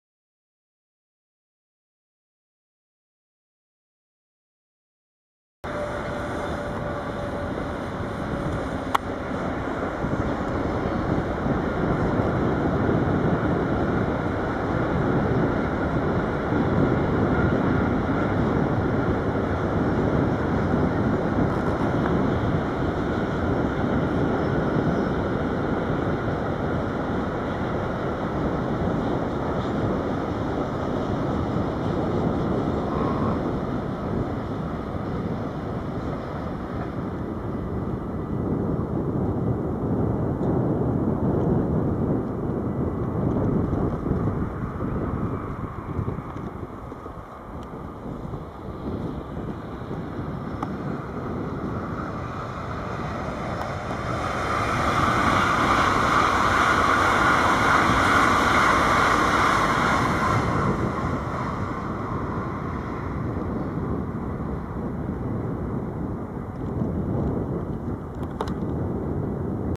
189 031 met kolen- of ertstrein passeert op snelheid Kijfhoek, naar de Maasvlakte, 2-3-2012
The electric locomotive ES64F4 is family of the "Eurosprinter" produced by Siemens Mobility.
It can be used in four different currentsystems, which are in use in Europe.
max speed 90-95 miles/hour
25 kV, 50 Hz ~ AC like in Germany
1,5 kV =           DC like in Holland
3 kV =              DC like in France

Die Elektrolokomotive ES64F4 ist eine Variante der EuroSprinter-Lokfamilie von Siemens Mobility (vormals Siemens Transportation Systems) mit Vier-Stromsystem-Ausstattung. In Deutschland ist sie vor allem als Baureihe 189 der Deutschen Bahn AG im Einsatz.